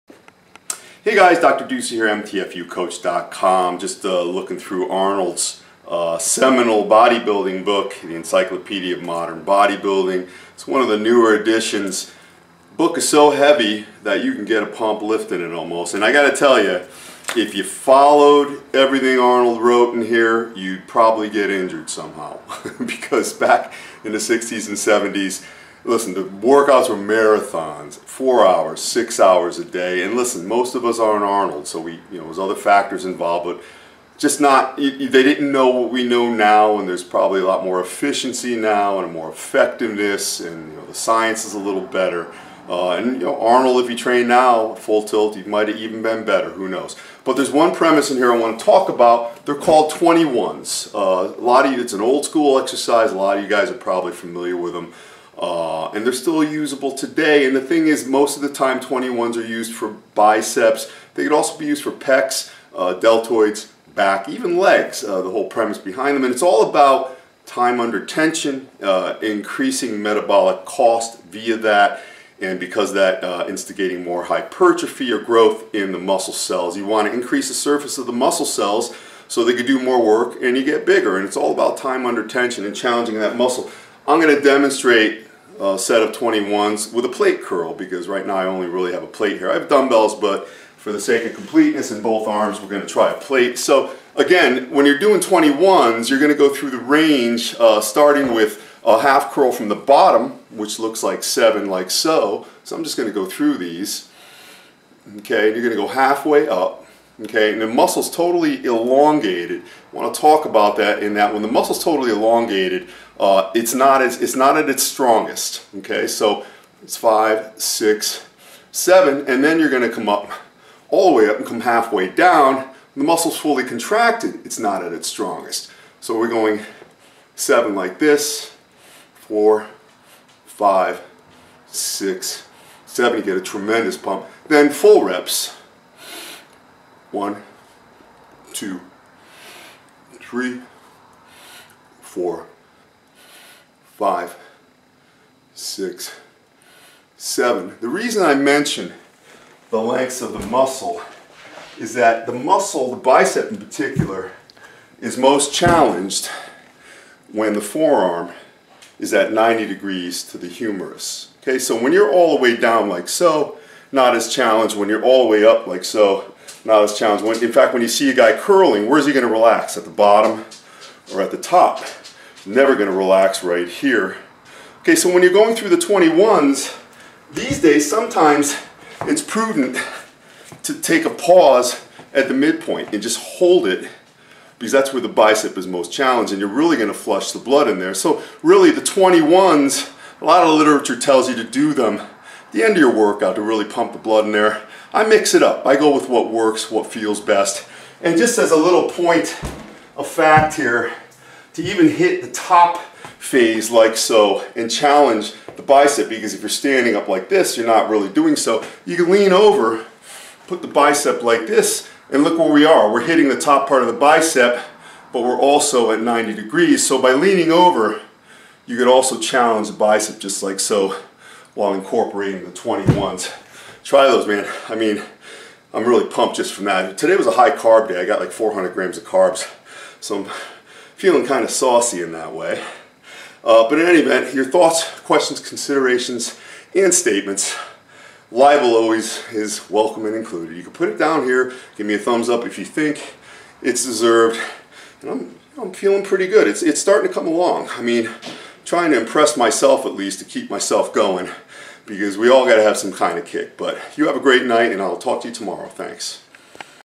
TRY THESE "21'S" FOR THE MOST INCREDIBLE PUMP!
It is a bodybuilding truth that if you were to try and replicate Arnold's marathon, 4 to 6 hour daily workouts from the 60's and 70's, you'd either get no results, injured, or both.

However, the Austrian Oak did, of course, build one of the greatest bodies in Human History. One of the techniques he did pound away at and benefit from were "21'S."

21'S are an old-school muscle-building technique that use partial reps and a long time-under-tension to overload the muscle.

While the old system certainly worked, playing around with 21'S to spend more time in the mid-range is more effective. indeed...this is where the muscle is strongest.
21'S can be successfully applied to exercises for the chest, shoulders, and back. and I will produce videos demonstrating such for each body part very soon.